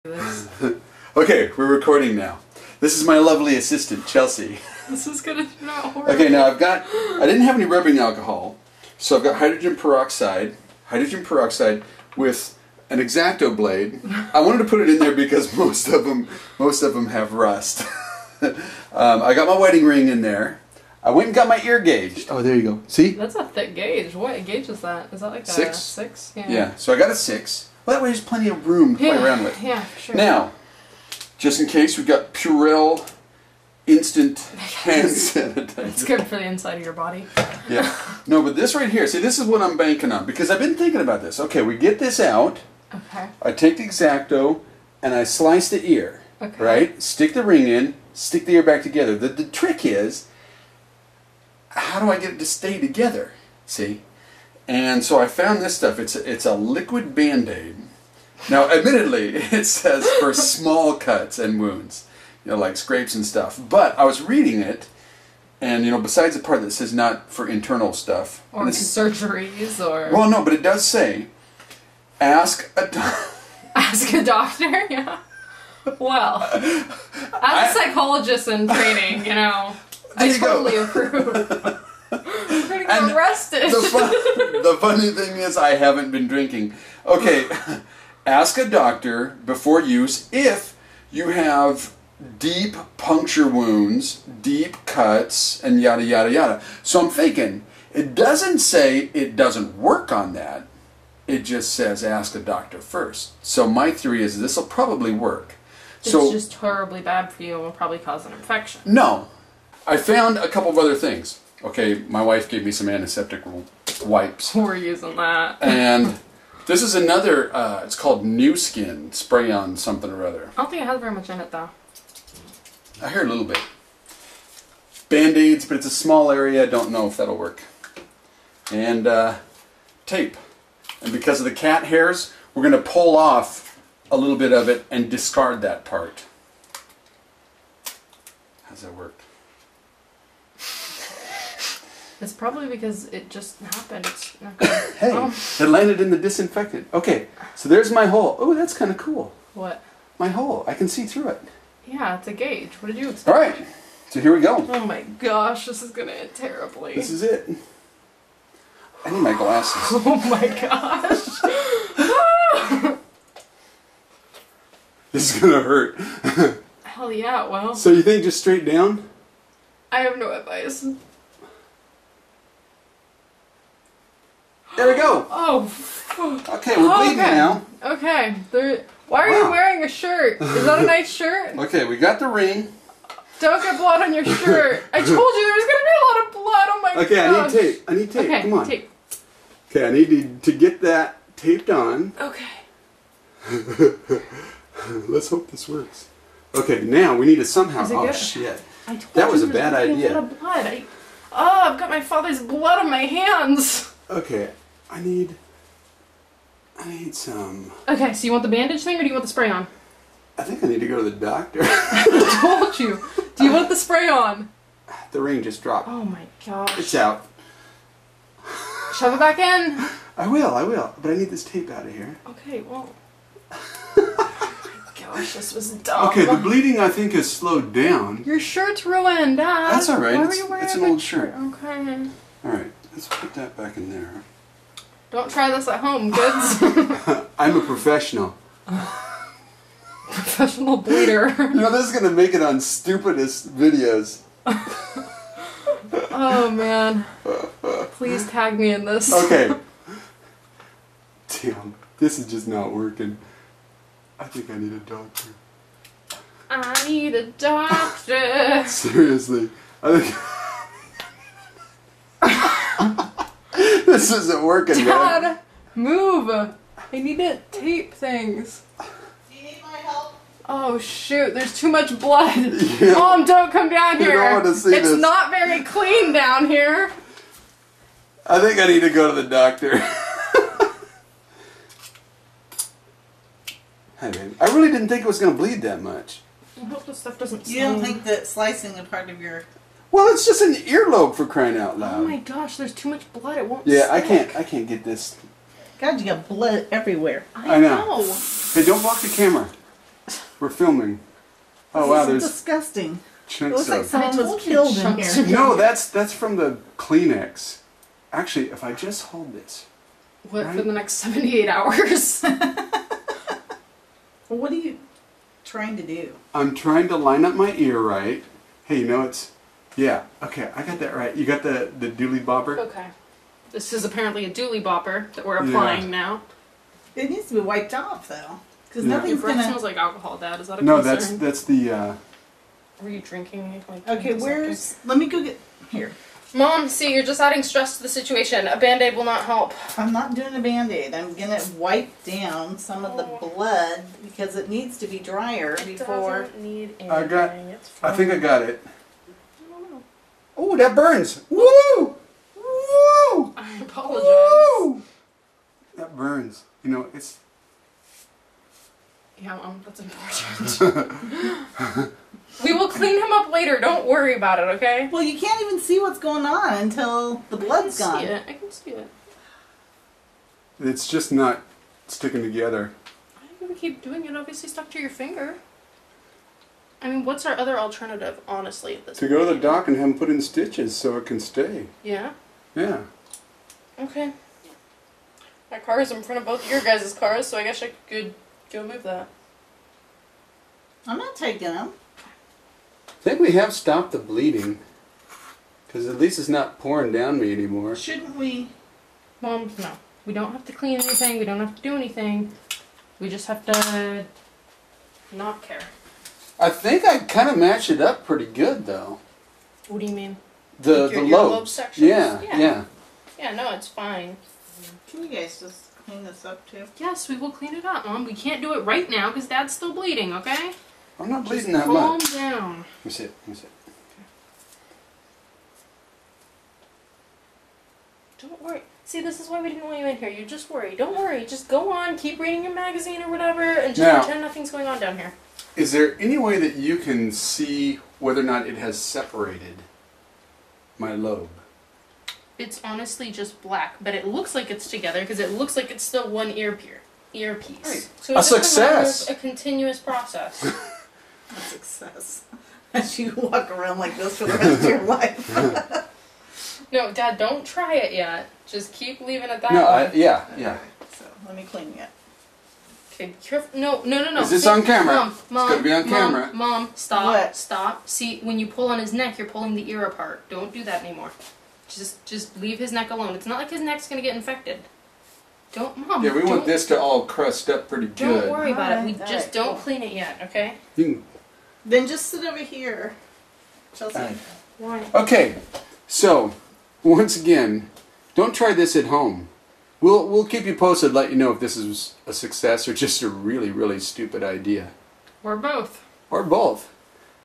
My Permanent Earring
Turning my wedding ring into a permanent earring.